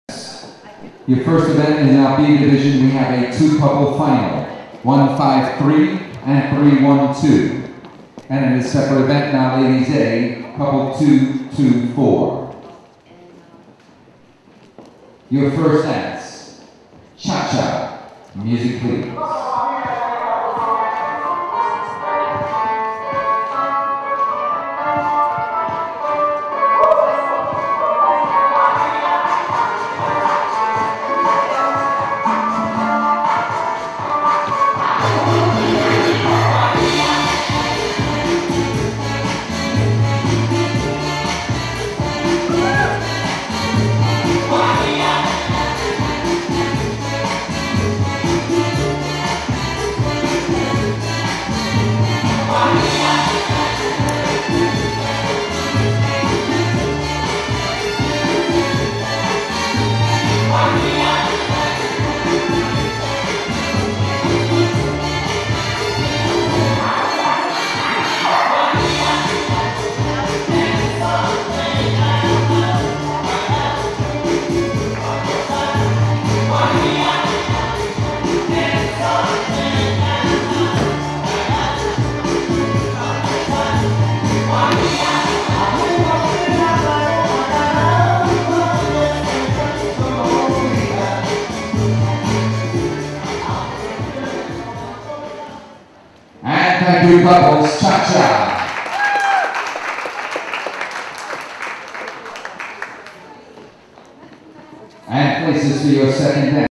Lauren Canton & Edgar Osorio - ESDC 2012 Cha Cha
Empire State Dancesport Championships - August 2012
Pro/Am Open Rhythm A1
Cha Cha